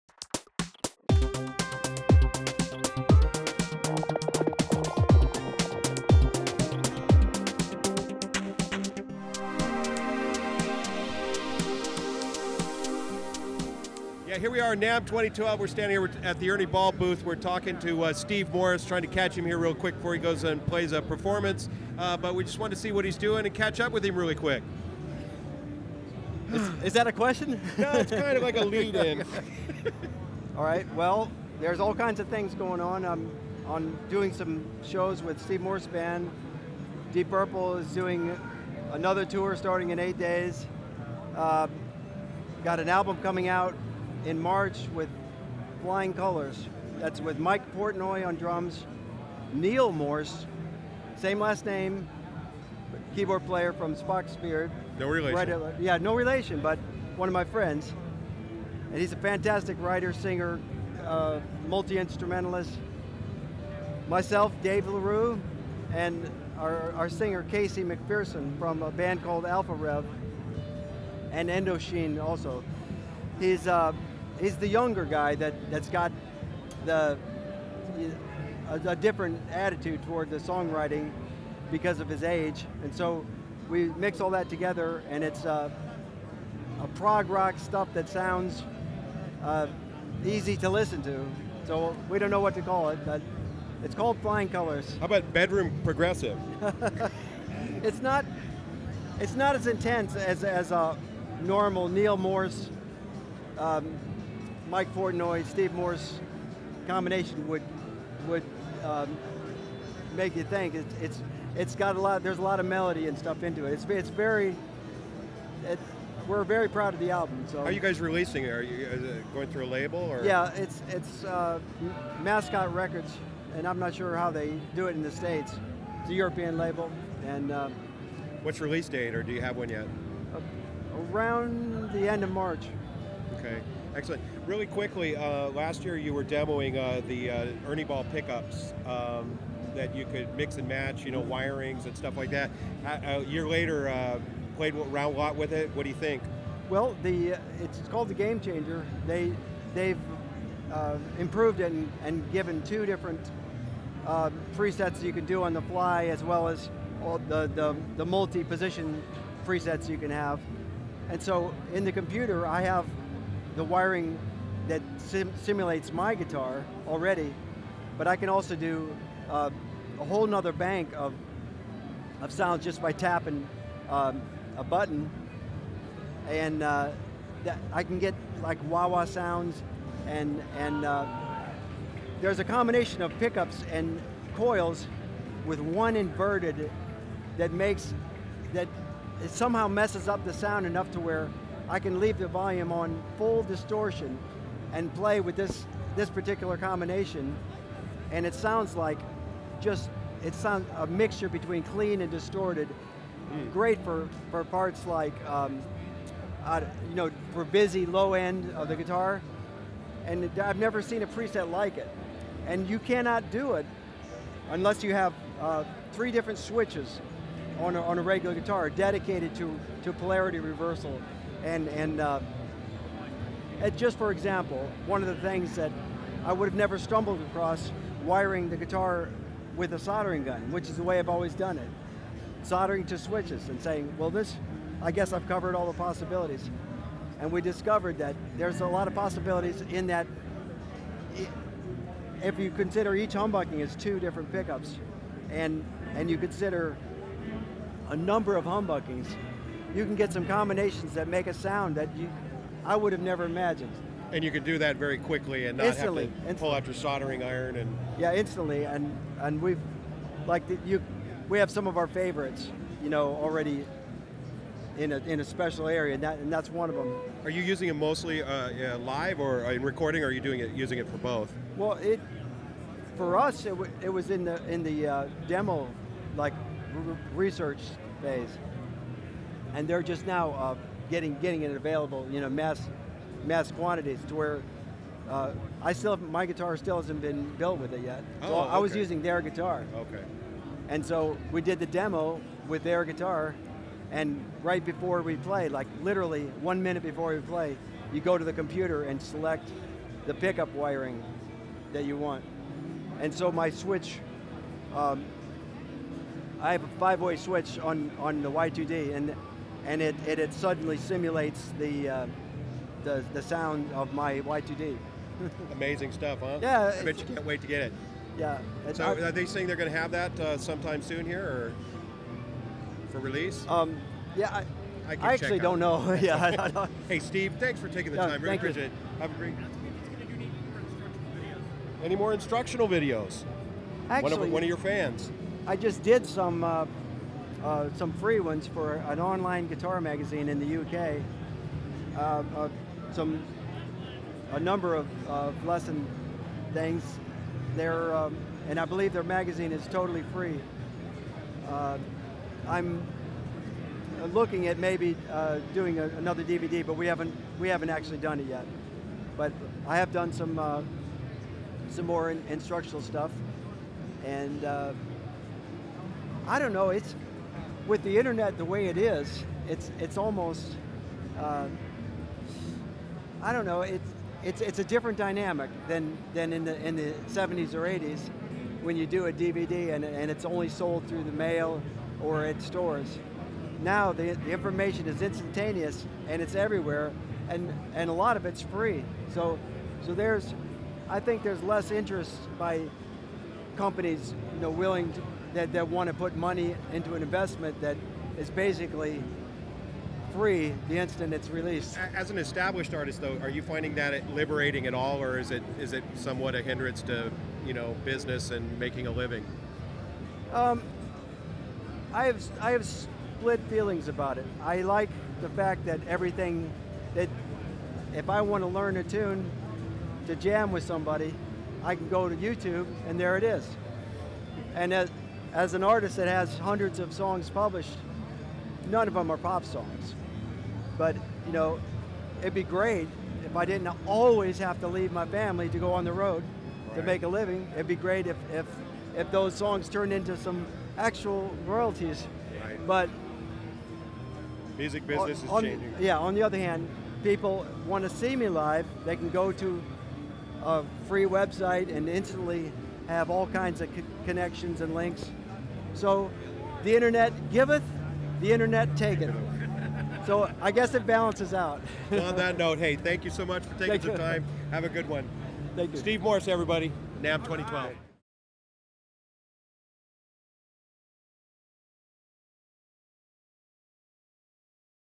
Steve Morse Interview Namm 2012- Musicianews
We caught up with Steve at the Namm show and he gave us his thoughts on music and the internet,